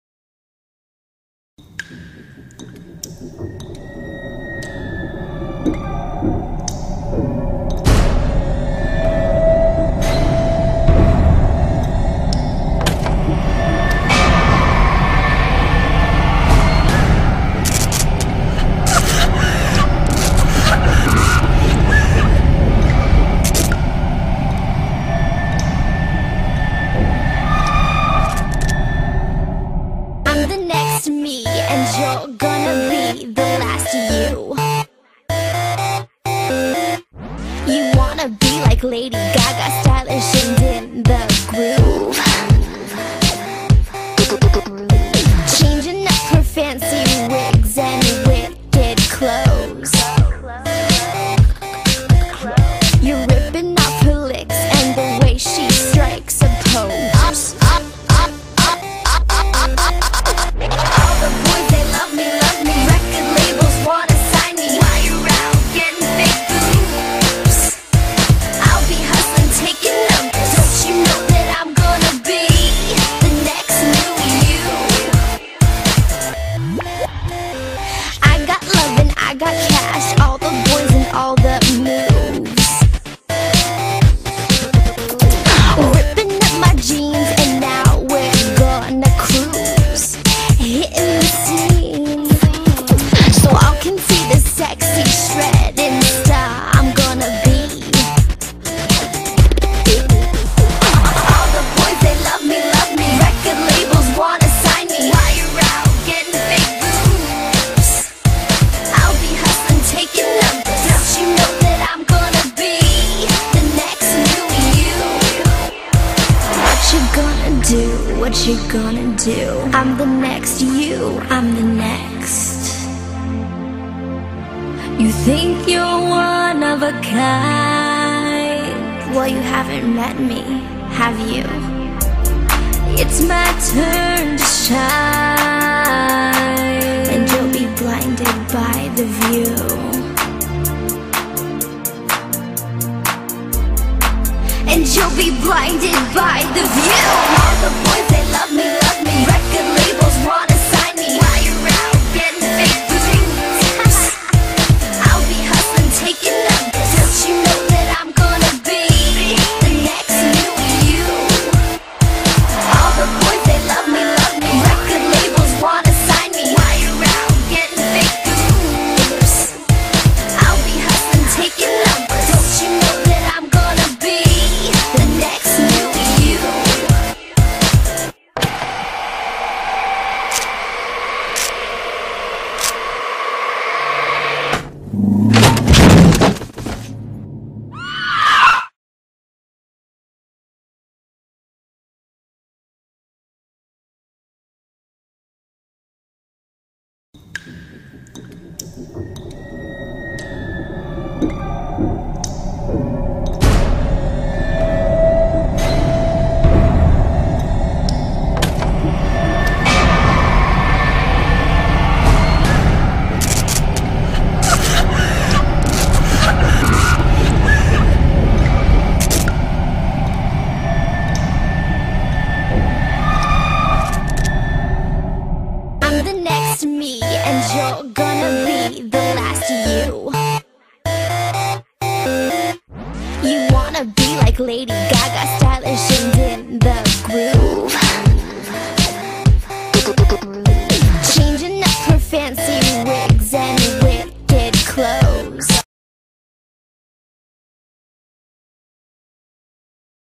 EXCLUSIVE Jackson Rathbone TROIX Mag Pictures
All The Pictures From TROIX Mag song is by Keana who is pictured with Jackson